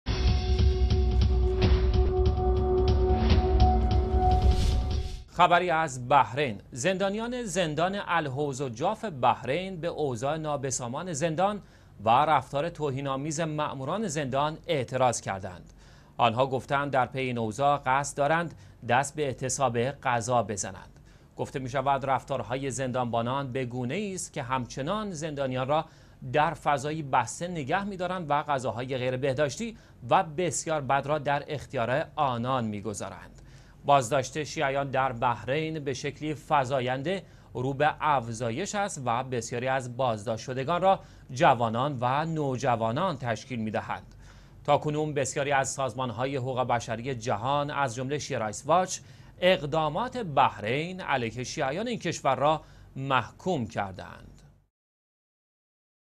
اعتراض زندانيان بحرينى به شکنجه و رفتارهاى توهين آميز ماموران حکومتی / ۵ مرداد ۱۳۹۵
اعتراض زندانيان بحرينى به شکنجه و رفتارهاى توهين آميز ماموران حکومتی

زندانیان زندان «الحوض الجاف» بحرین به اوضاع نابسامان زندان و رفتار توهین آمیز ماموران زندان اعتراض دارند.

آنها گفته اند در پى اين اوضاع می خواهند دست به اعتصاب غذا بزنند.

گفته مى شود رفتارهای زندان بانان به گونه ای است که همچنان زندانیان در فضایی بسته بوده و غذاها نیز غیربهداشتی و بسیار بد است.

بازداشت شیعیان در بحرین به شکل فزاینده ای رو افزایش گذاشته و بسیاری از بازداشت شدگان را جوانان و نوجوانان تشکیل می دهند.

تاکنون بسیاری از سازمانهای حقوق بشری جهان، از جمله شیعه_رایتس_واچ (Shia_Rights_Watch) اقدامات بحرين علیه شیعیان اين كشور را محکوم نموده اند.

الثلاثاء - ٢١ شوال ١٤٣٧
Tuesday - 2016 26 July